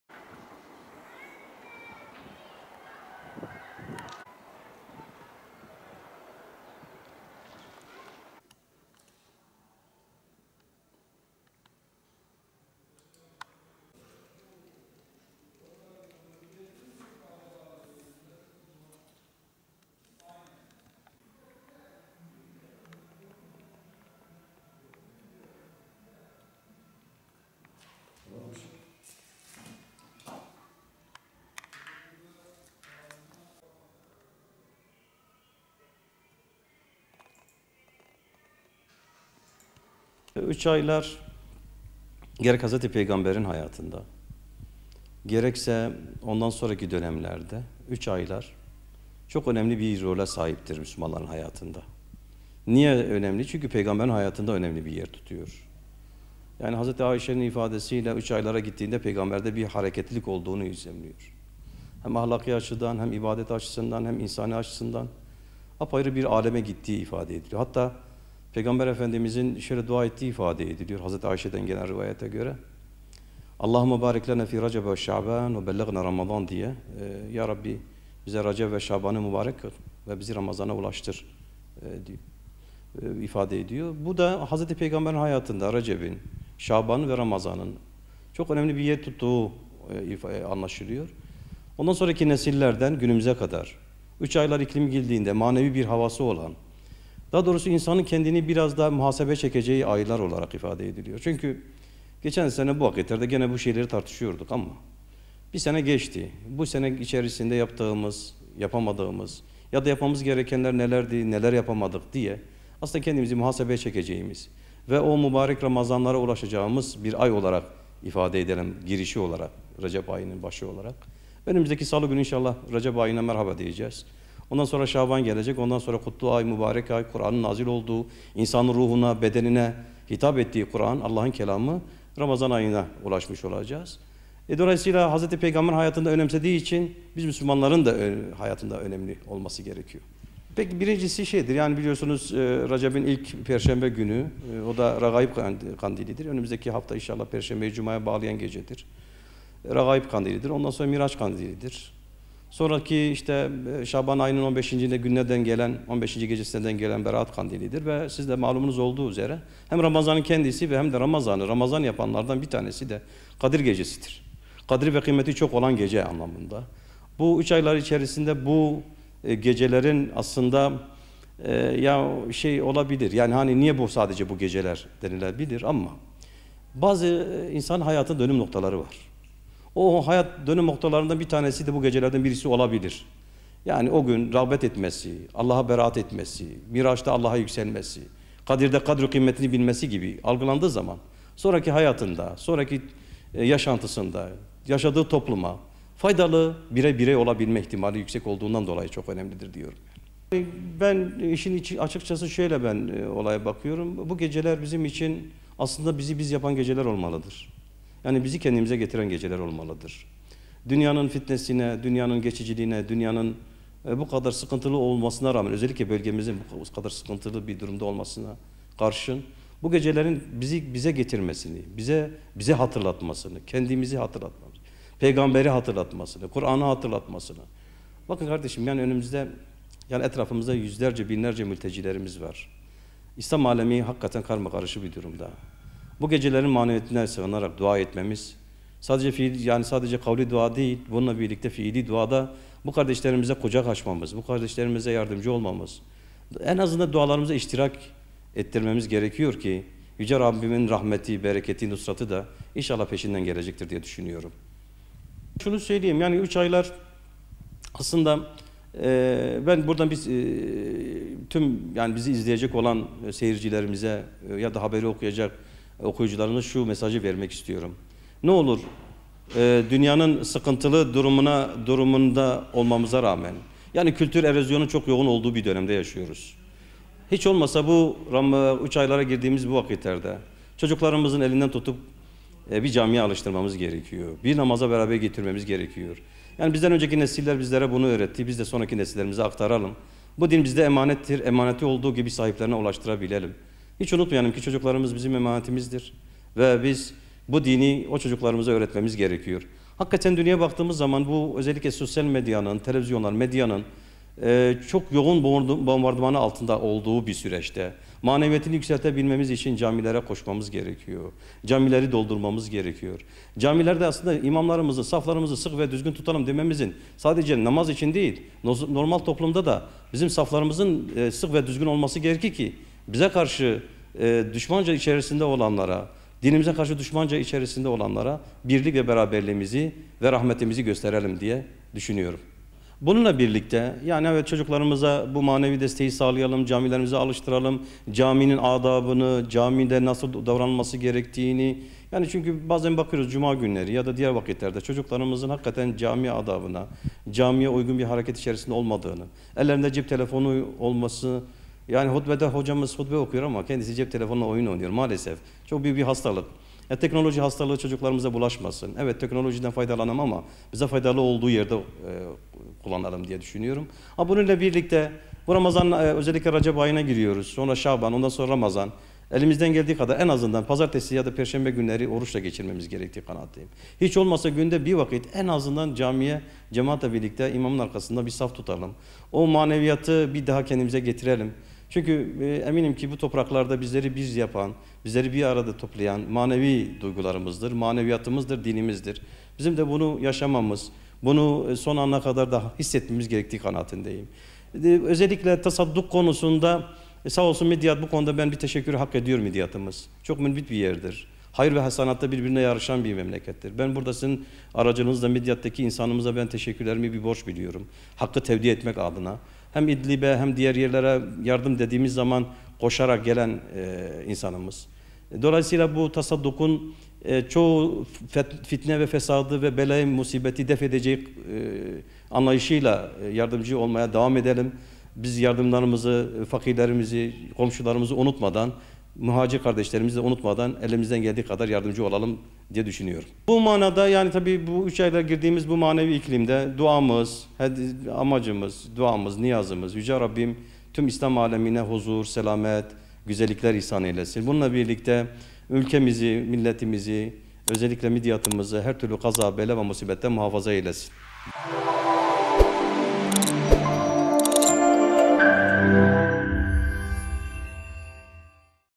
Midyat Müftüsü Mehmet Şafi Bilik: "Üç aylar muhasebe yapmak için bir fırsattır"
​Üç ayların başlamasıyla kutlu bir mevsime girildiğini söyleyen Midyat ilçe Müftüsü Mehmet Şafi Bilik, bu ay ve gecelerin muhasebe için bir fırsat olduğunu ve verimli bir şekilde değerlendirilmesi gerektiğini belirtti.

Değerli İzleyicilerimiz;

TÜRKÇE

KÜRTÇE-KURMANCI

ARAPÇA

İNGİLİZCE

FARSÇA

KÜRTÇE-SORANİ